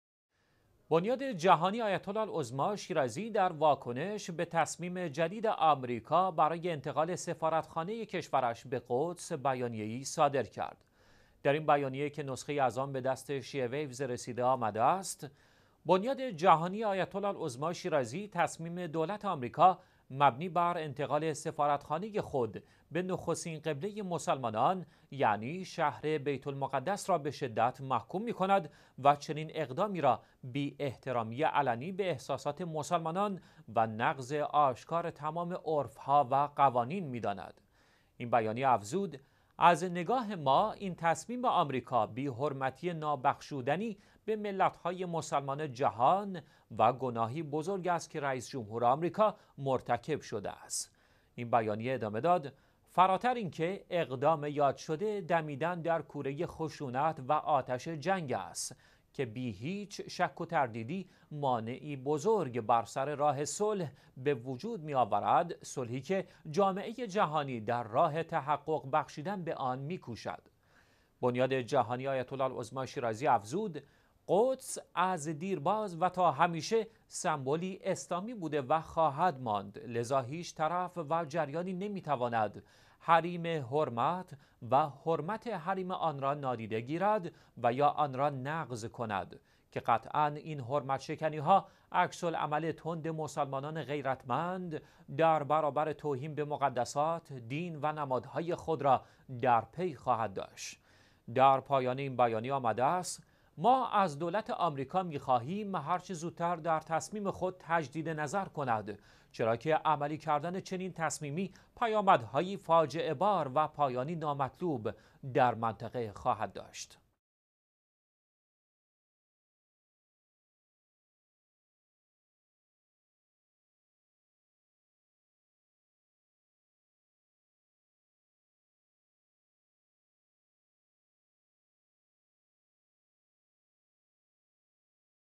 بیانیه بنیاد جهانی آیت الله العظمی شیرازی در واکنش به تصمیم جدید آمریکا
بنیاد_جهانی_آیت_الله_العظمی_شیرازی، در واکنش به تصمیم جدید آمریکا، مبنی بر انتقال سفارتخانه کشورش به قدس بیانیه ای صادر کرد.

در این بیانیه که نسخه از آن به دست شیعه ویوز رسیده آمده است:

بنیاد جهانی آیت الله العظمی شیرازی نسبت به تصمیم دولت آمریکا مبنی بر انتقال سفارتخانه خود به نخستین قبله مسلمانان، یعنی شهر بیت_المقدس را به شدت محکوم نموده و چنین اقدامی را بی احترامی علنی به احساسات مسلمانان و نقض آشکار تمام عرف ها و قوانین می داند.

این بیانیه افزود:از نگاه ما تصمیم آمریکاییِ مورد نظر، بی حرمتیِ نابخشودنی به ملت های مسلمانِ جهان و گناهی بزرگ است که رئیس جمهور آمریکا به آن دست زده.

این بیانیه ادامه داد: فراتر این که اقدام یادشده دمیدن در کوره خشونت و آتشِ جنگ است که بی هیچ شک و تردیدی مانعی بزرگ بر سر راه صلح به وجود می آورد؛ صلحی که جامعه جهانی در راه تحقق بخشیدن به آن می کوشد.

بنیاد جهانی آیت الله العظمی شیرازی، افزود: قدس از دیرباز و تا همیشه سمبلی اسلامی بوده و خواهد ماند، لذا هیچ طرف و جریانی نمی تواند حریمِ حرمت و حرمت حریم آن را نادیده گرفته و یا آن را نقض کند که قطعاً این حرمت شکنی ها عکس العمل تند مسلمانان غیرتمند را نسبت به مقدسات، دین و نمادهای خود در پی خواهد داشت.

در پایان این بیانیه آمده است: ما از دولت آمریکا می خواهیم، هرچه زودتر در تصمیم خود تجدیدنظر کند؛

چرا که عملی کردن چنین تصمیمی پیامدهایی فاجعه بار را در منطقه رقم زده و پایانی نامطلوب خواهد داشت.

دوشنبه - ٢٠ آذر ۱۳۹۶
الإثنين - ٢٢ ربيع الاول ١٤٣٩
Monday - 2017 December 11